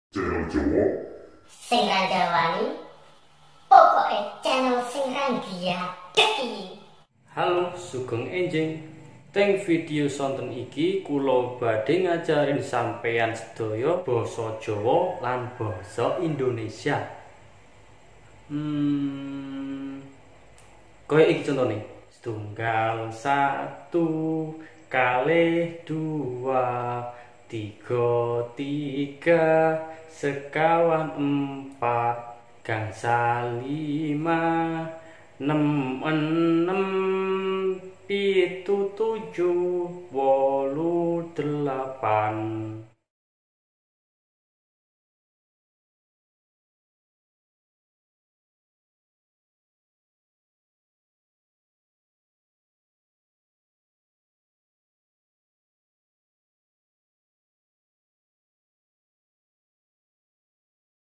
belajar berhitung || Jawa indonesia
Belajar bahasa jawa ke bahasa indonesia, khususnya untuk kalian yang belum begitu paham tentang bahasa jawa